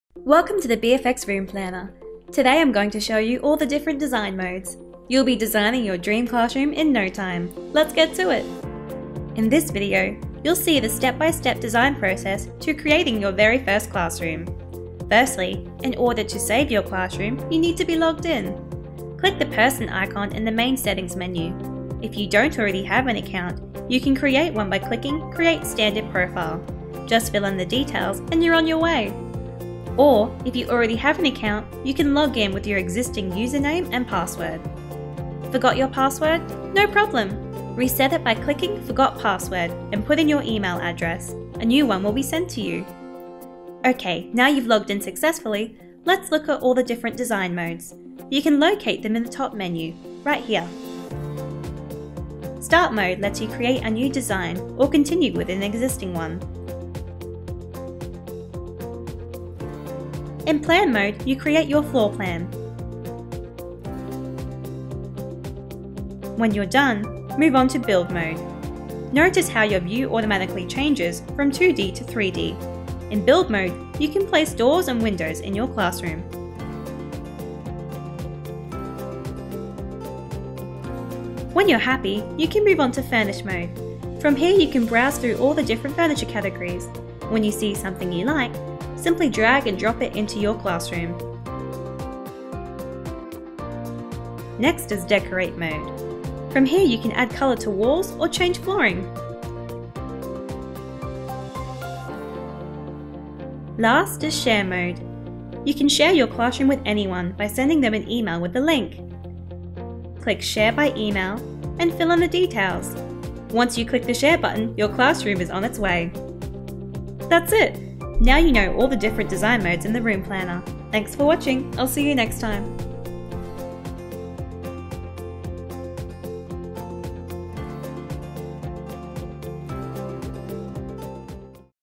BFX Roomplanner Lesson #1 - Step based Design Flow
In this video you will see the step by step design process to create your very first classroom in the BFX 3D Room Planner!